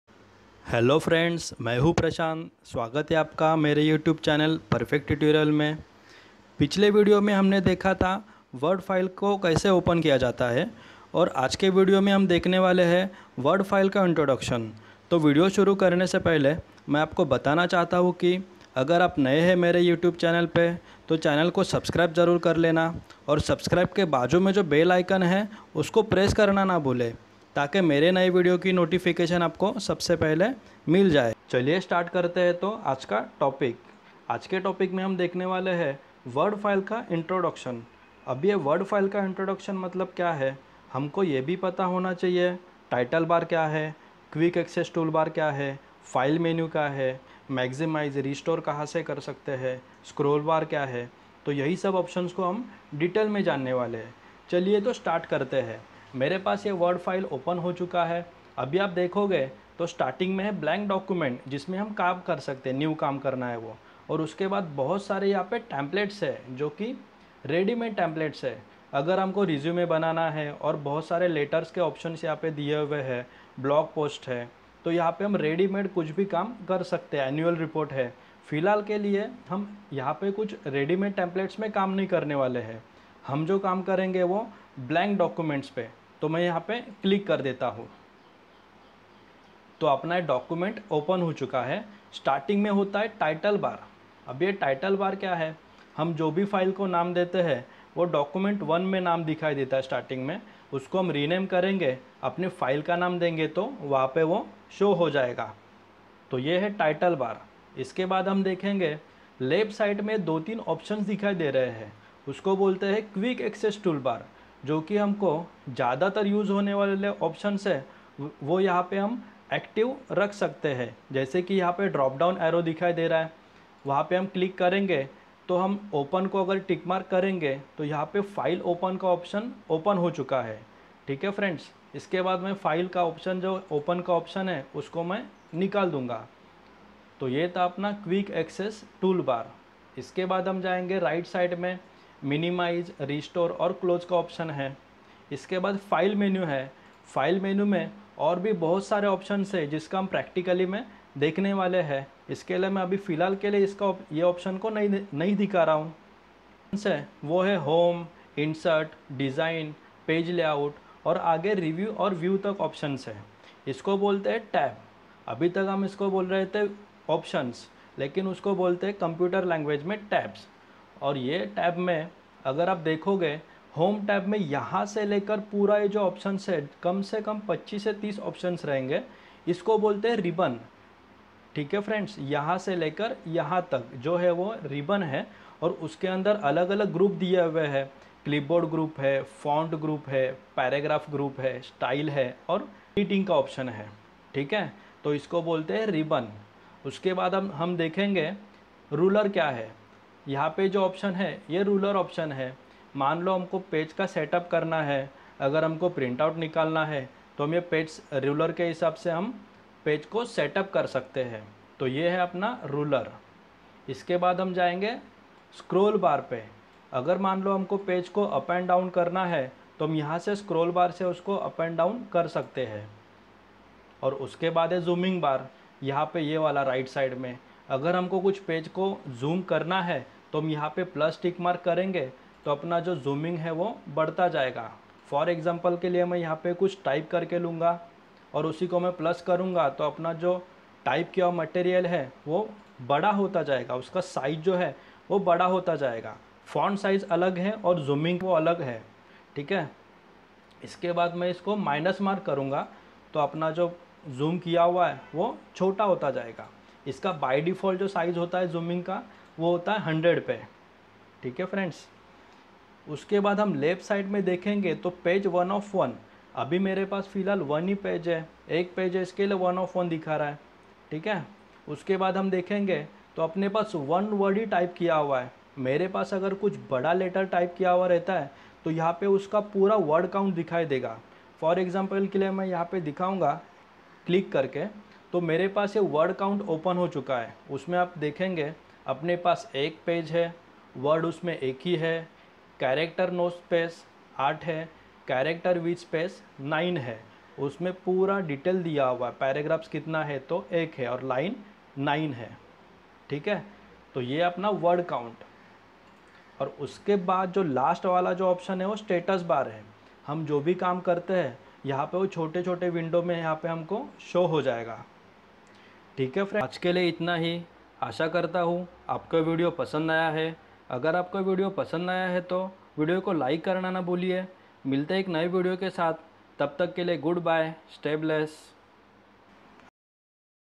M/S Word Introduction In Hindi. M/S Word का Introduction. M/s Word Hindi Tutorial. For Beginners.
Hello Friends,

Microsoft Word In Detail Part - 2

Is Video Se Hum M/S Word Ki Series Start Karnewale Hai Jo Basic To Advance Raehga. Tho Aaj Ke Video Me Hum Dekhnewale Hai Word Ka Introduction. Is Introduction Me Humne Home Tab Se Lekar View Tab Tak Sabhi Tabs Ka Explanation Kiya Hai. Jo Ekdum Saral Bhasha Me Hai. M/S Word 2007, 2010, 2013, 2016, 2019 Ye Sabhi Version Ka Introduction Same Hai. Ye Video Beginners Ke Liye Helpful Hai Jo Word Sikhne Chahte Hai.

Perfect Tutorial